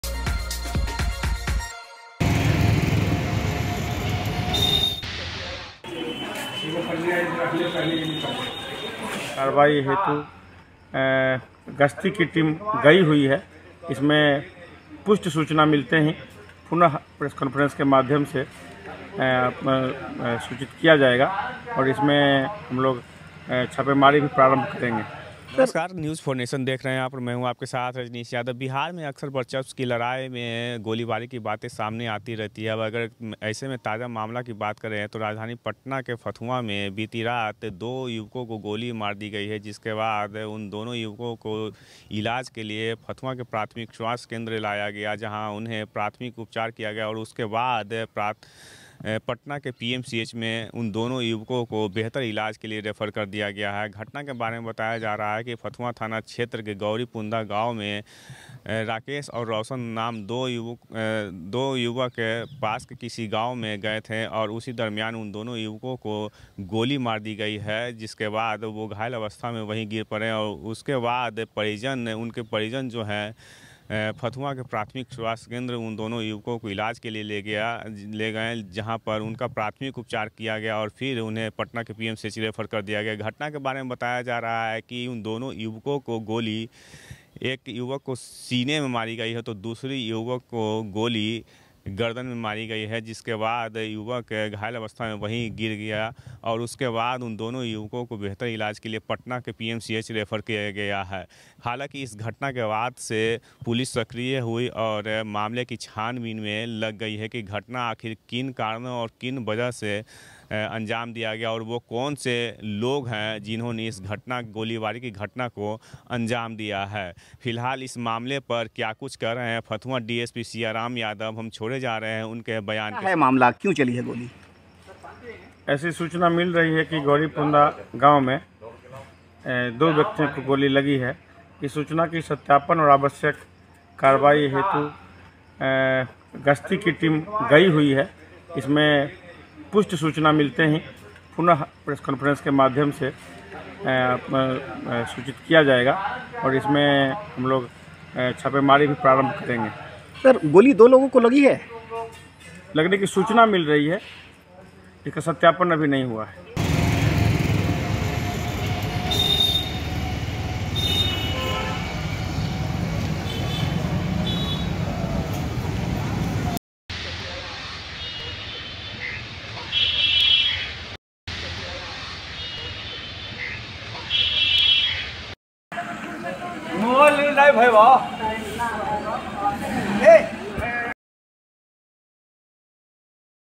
Patna City में फिर वर्चस्व की ल/ड़ाई, गांव मे दूसरे टोले में गए दो युवकों को बदमाशों ने ठोका
Patna City में फिर वर्चस्व की ल/ड़ाई, गांव मे दूसरे टोले में गए दो युवकों को बदमाशों ने ठोका | Bihar News| News4Nation